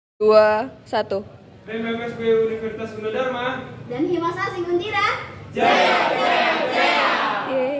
HIMA Sasing UDN & BEM FSB UG
Video ini berasal dari salah satu mahasiswa Prodi Sastra Inggris Undira yang ikut dalam Studi Banding dengan BEM FBS Universitas Gunadarma.